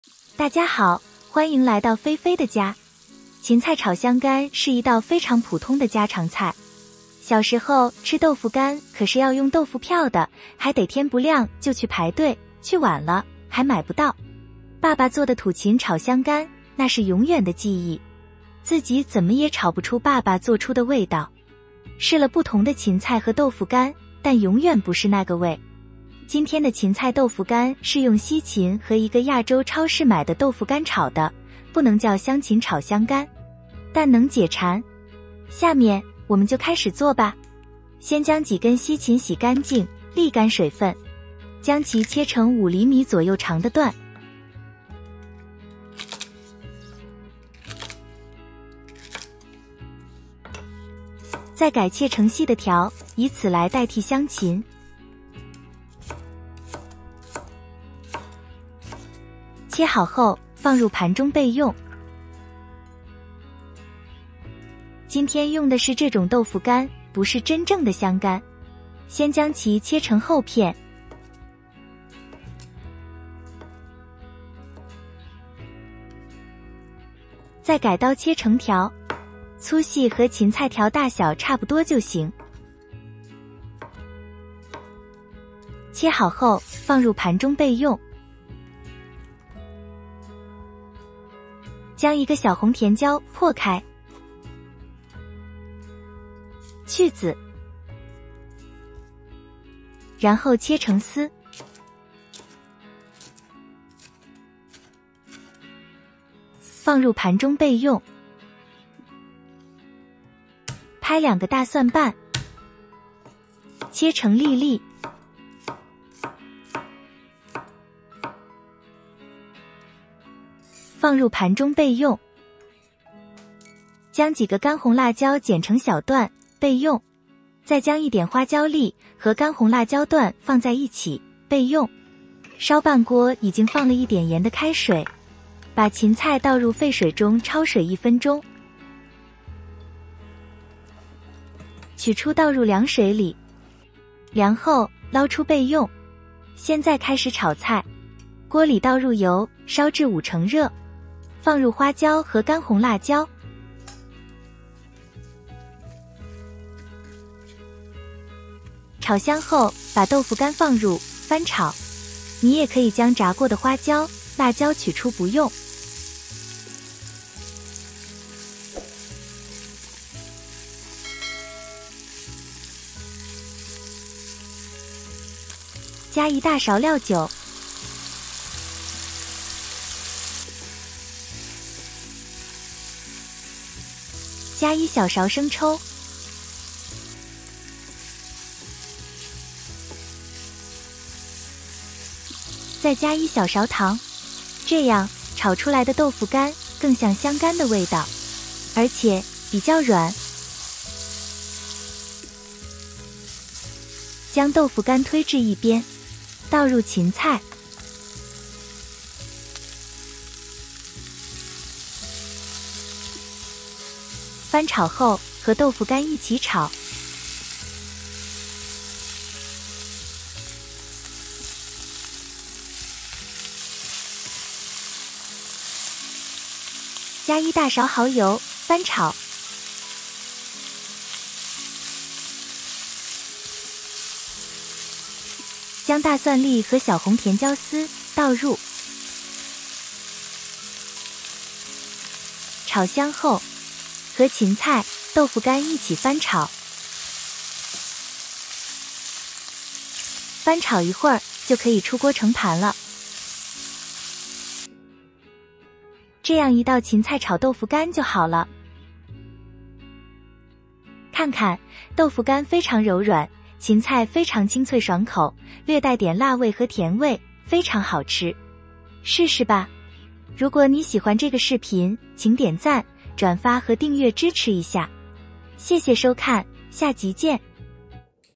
Stir-fried Celery with Dried Tofu（芹菜豆腐干）- 清香爽口，营养健康，有利于降血压的最佳食物
芹菜炒豆腐干是一道极为普通的家常小菜。小时候，家里都用土芹和香干来做这道菜，那土芹的药香味和豆腐干的酱香味，让人回味无穷，绝对的家乡的味道。现在的芹菜和豆干好像没有那个味道。但其营养价值还是很丰富的。因为简单易做，所以我们家经常吃。

Ingredients:
     5 Celery Sticks - 芹菜杆 5个
     1 Dried ToFu - 豆腐干 1包
     1 red sweet pepper - 小红甜椒 1个
     2 Garlic Cloves - 大蒜瓣 2个

Seasoning:
     1 tbsp Oyster Sauce - 蚝油 1 tbsp
     1 tsp Light Soy Sauce - 生抽 1 tsp
     1 tsp Sugar - 糖 1tsp
     1 tbsp Cook Wine - 料酒 1 tbsp